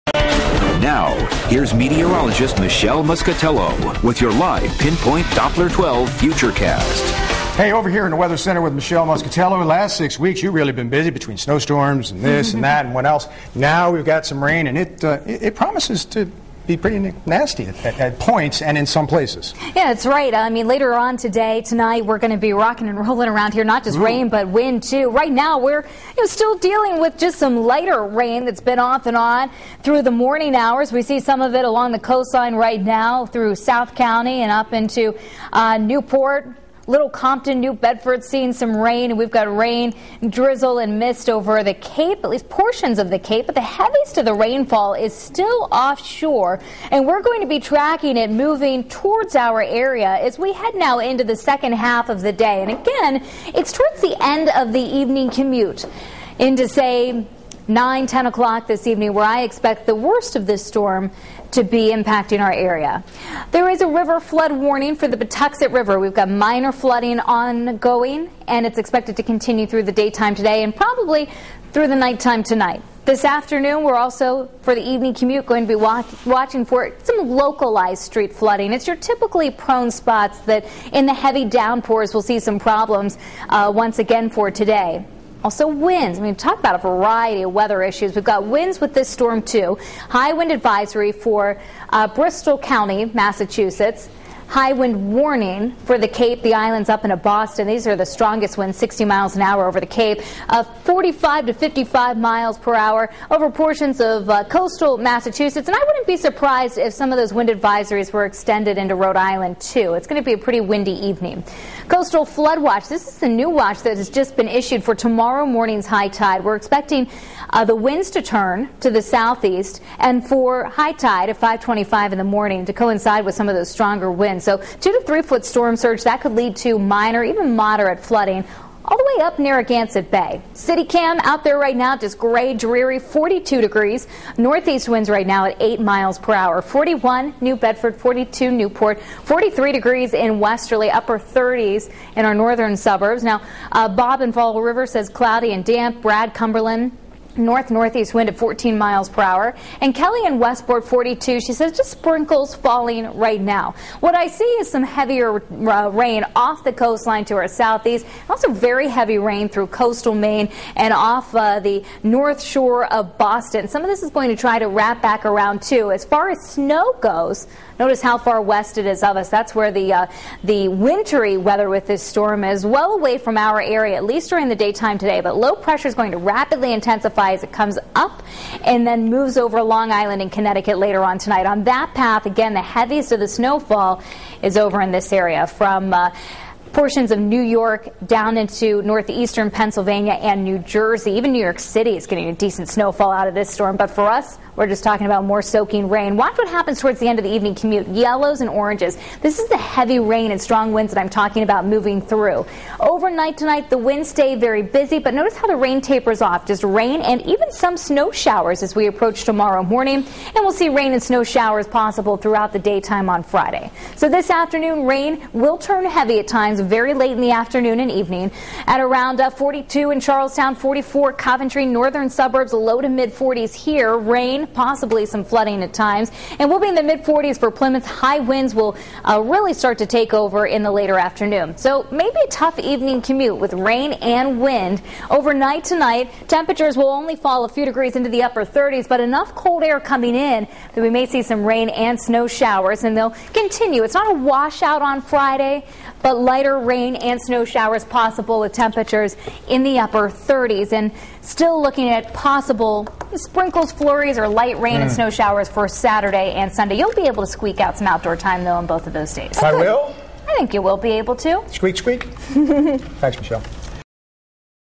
Weather at 12
A rainstorm is expected to impact the area on Thursday.